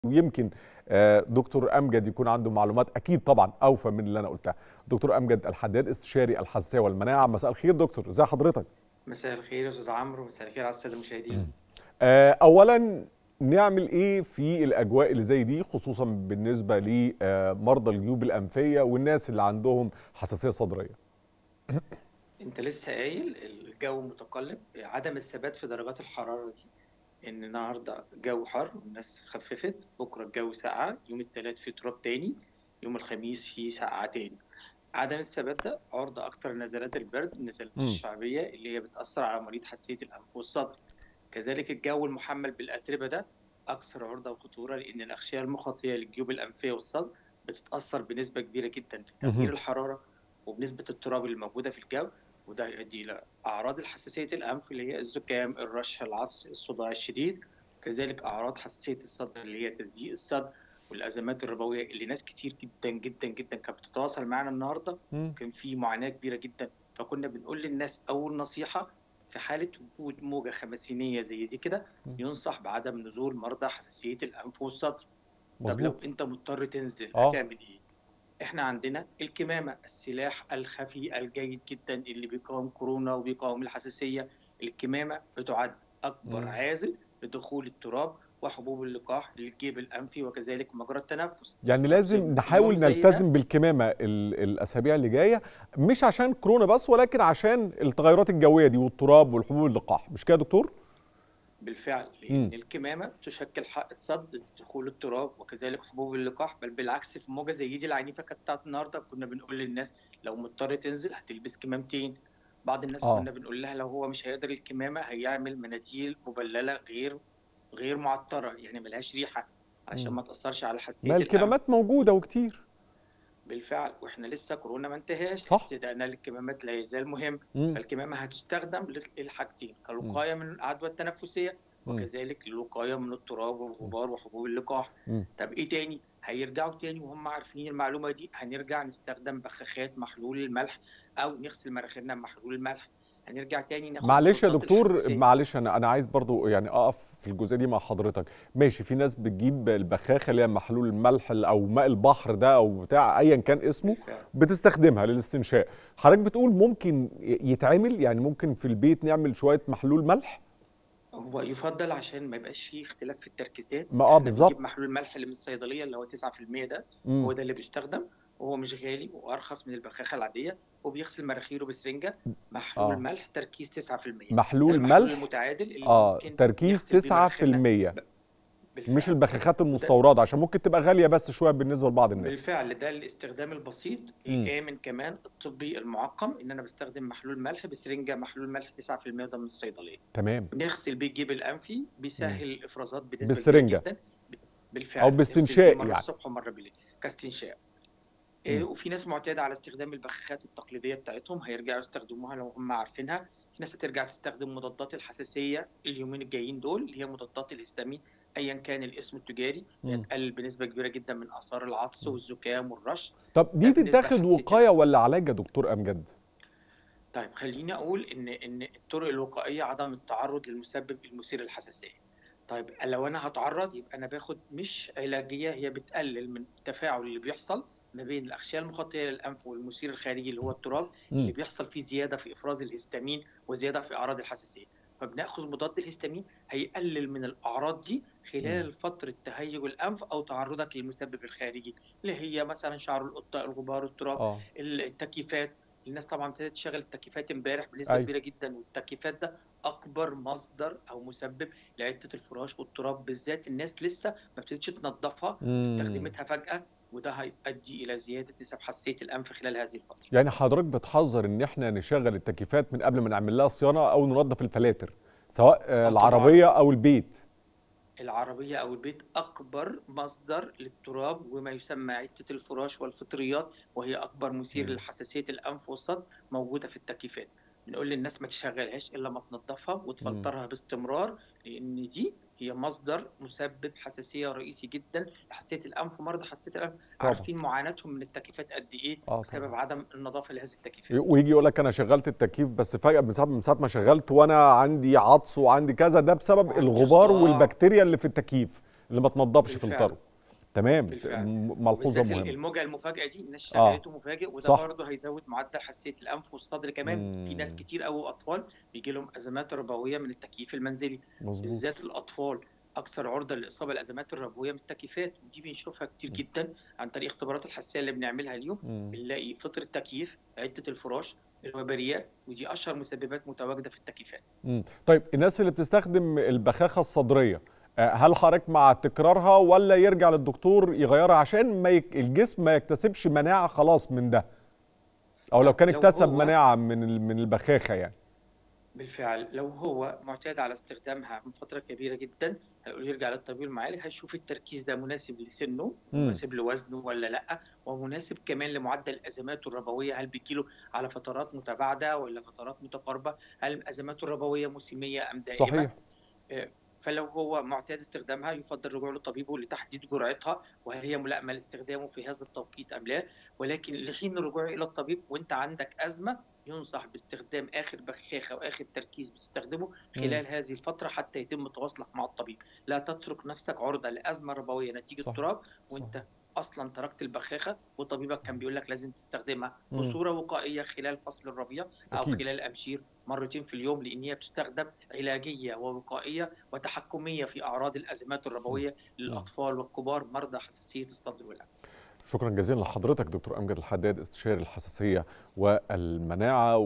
د. أمجد الحدادي يوجه تحذيرا شديد اللهجة لمرضى الجيوب الأنفية بسبب تقلبات الجو
من مصر | د. أمجد الحدادي يوجه تحذيرا شديد اللهجة لمرضى الجيوب الأنفية بسبب تقلبات الجو